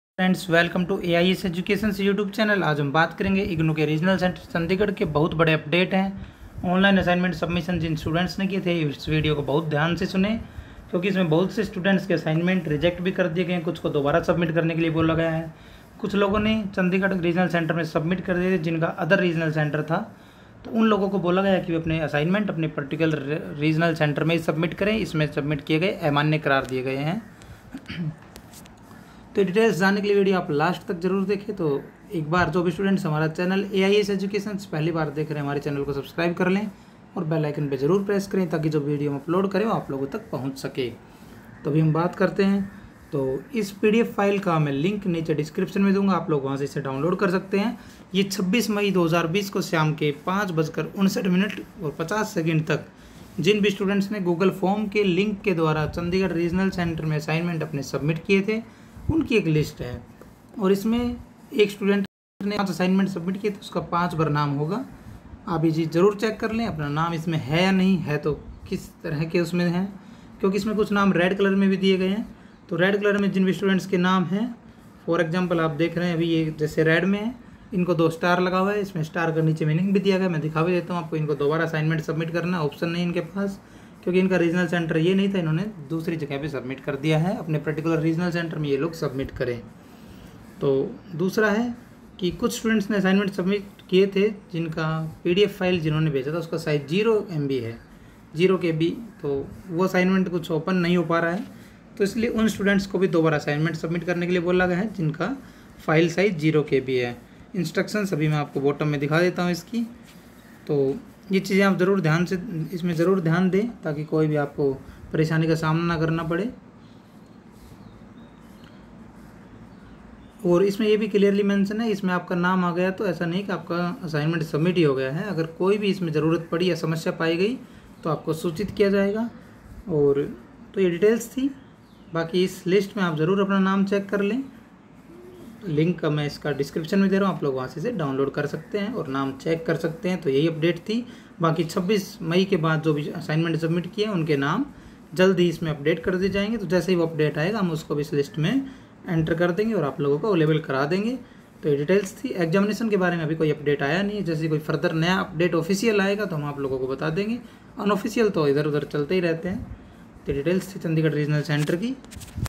IGNOU ASSIGNMENT SUBMISSION STATUS CHANDIGARH REGIONAL CENTRE
This video gives detailed explanation on assignment submission status of regional center CHANDIGARH of ignou. This contains list of all students who submitted assignments by google form link hy 26 MAY 2020 BY 05:59:50 PM. LIST WILL BE UPDATED LATER ON FOR STUDENTS WHO SUBMITTED AFTER 26 MAY 2020.

STUDENTS WHOSE NAME APPEARED RED WITH * IN LIST ARE ADVISED TO SUBMIT ASSIGNMENTS TO THEIR CORRECT REGIONAL CENTRES. THEIR ASSIGNMENTS ARE REJECTED BY CHANDIGARH REGIONAL CENTRE.